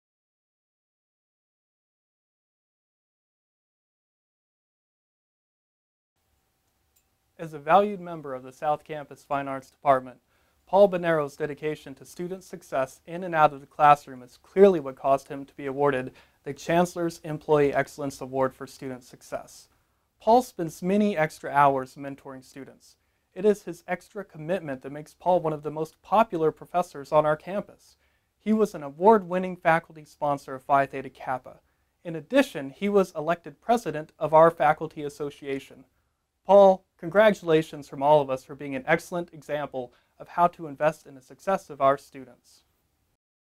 Student Success Congratulatory Video
Chancellor's Breakfast congratulatory video - Winner Paul Benero